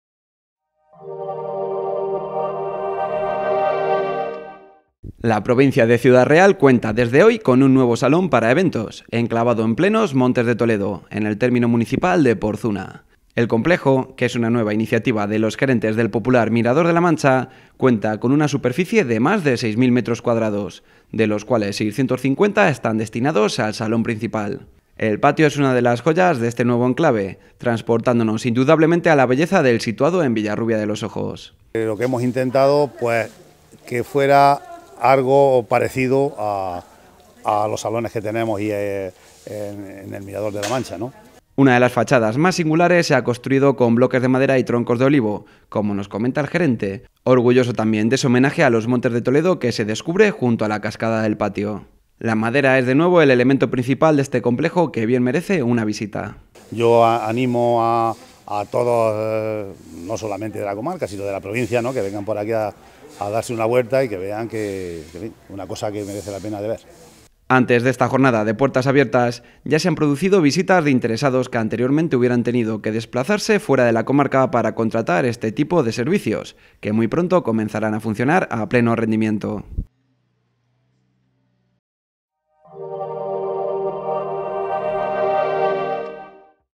Miramontes, un nuevo Salón de Eventos que nace en la provincia
La provincia de Ciudad Real cuenta desde hoy con un nuevo salón para eventos, enclavado en plenos Montes de Toledo, en el término municipal de Porzuna. El complejo, que es una nueva iniciativa de los gerentes del popular Mirador de la Mancha, cuenta con una superficie de más de 6.000 m2, de los cuales 650 están destinados al salón principal. El patio es una de las joyas de este nuevo enclave, transportándonos indudablemente a la belleza del situado en Villarrubia de los Ojos.